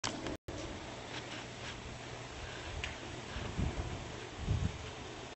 Snow Garden - After effects Creation
This video was made from 3 photos stitched together in photoshop. In After effects I added the position movement, camera shake, snow particle effects and audio from another youtube snow video.

Photos into video done :)
Thanks VideoCopilot!